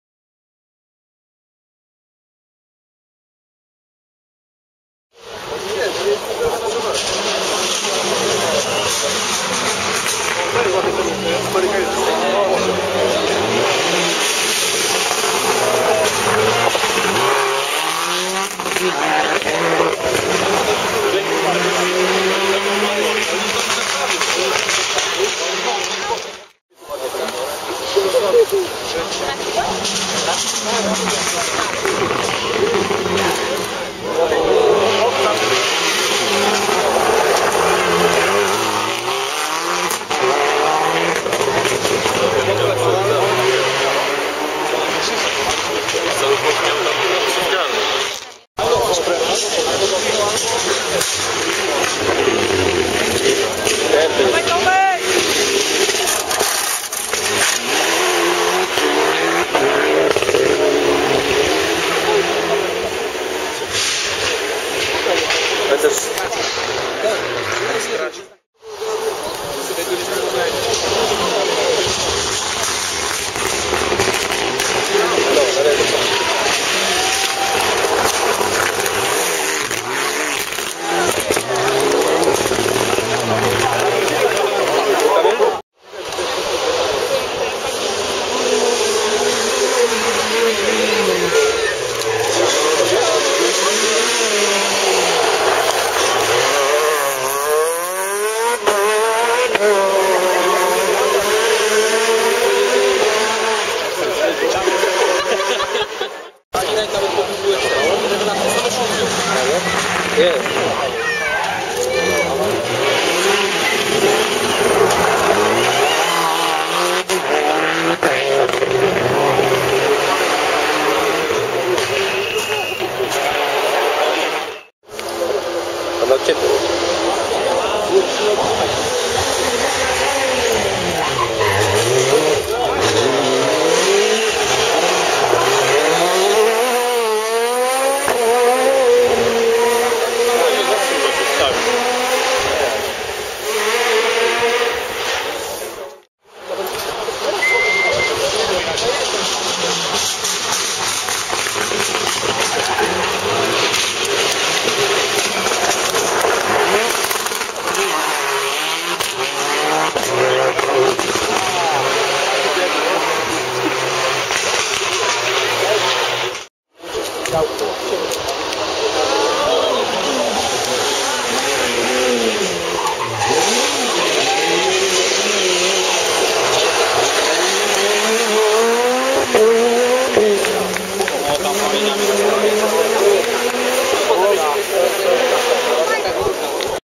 21 Rajd Dolnoslaski 2011 - Dlaczego Bryan Bouffier wygrywa w RSMP?
Krotkie porownanie. Za nagrania dzieki wielkie uzytkownikowi 11heilig!